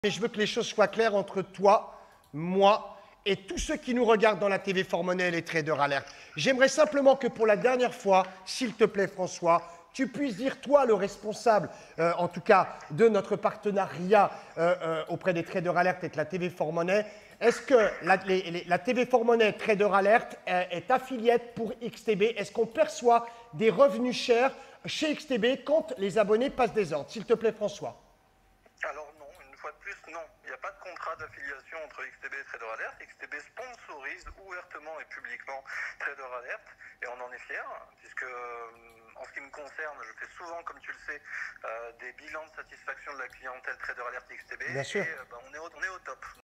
SCANDALE ! Thami Kabbaj Non tradersalertes n'est pas affiliate XTB
Thami Kabbaj affirme que tradersalertes perçoit des revenus quand les abonnés passent leurs ordres. La vérité en image.
La diffamation est un délit. Diffamation publique : 12 000 € (art. 23)
Diffamation publique envers une personne ou une institution publique : 45 000 € (art. 30 et 31) Diffamation publique raciale : 45 000 € (art. 32) €